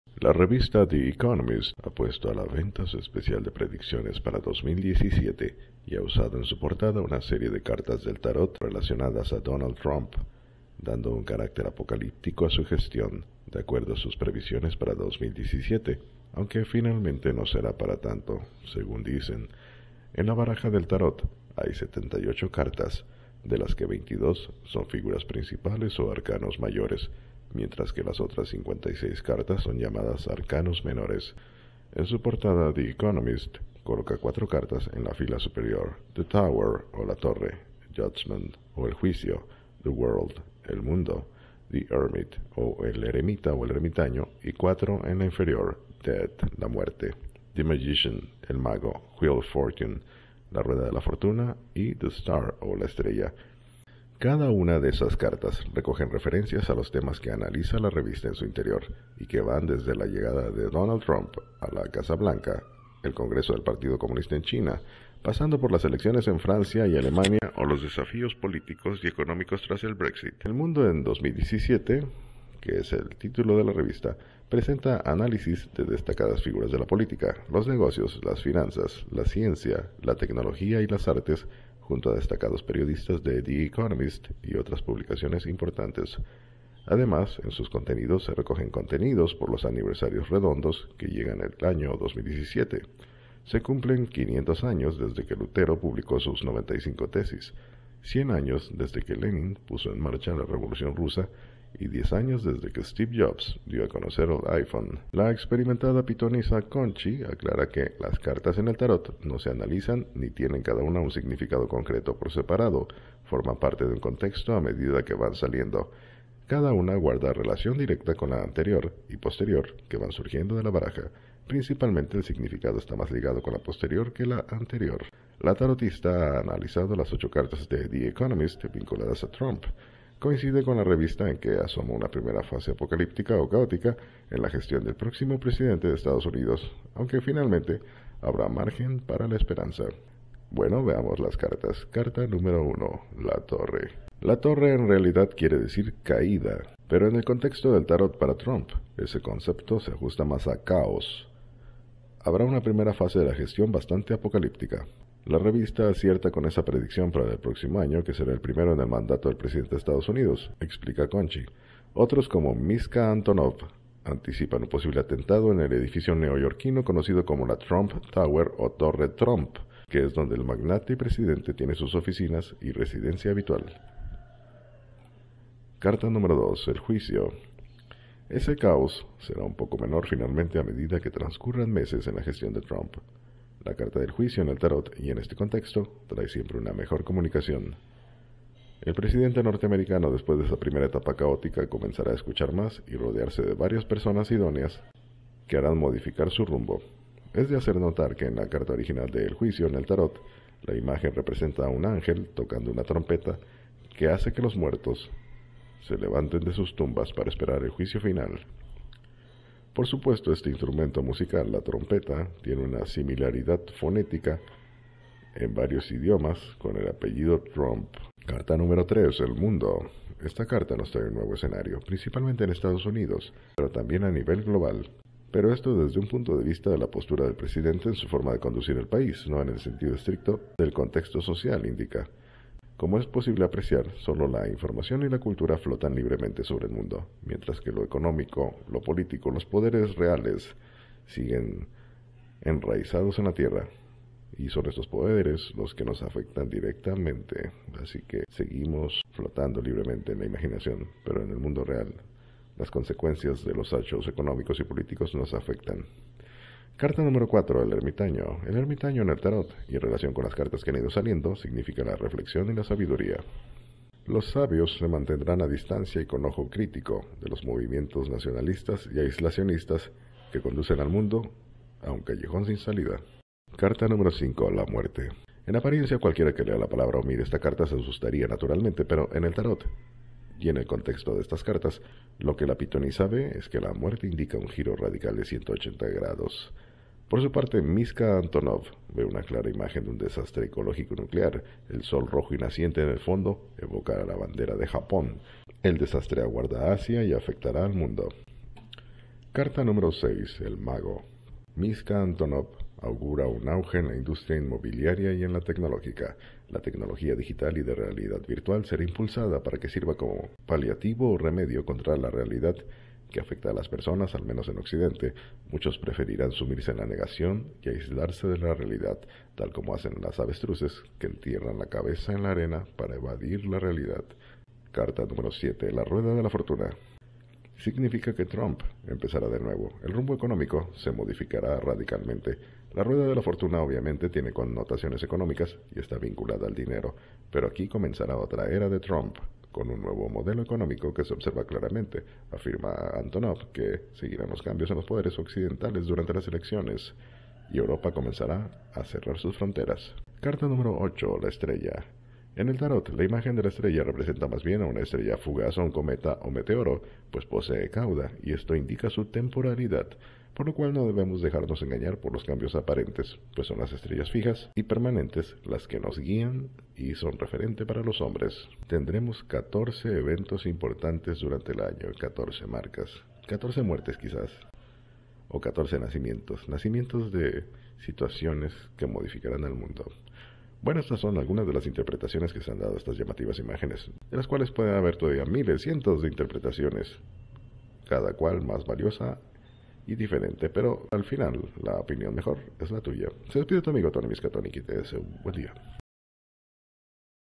Prediccionespara 2017 en portada revista The Economist. ¿Previeron el METAVERSO de Facebook EN 2017?
La revista The Economist ha puesto a la venta su especial de predicciones para el próximo año, y ha usado en su portada una serie de cartas del Tarot relacionadas a Donald Trump, dando un carácter apocalíptico a su gestión, de acuerdo a sus previsiones para 2017.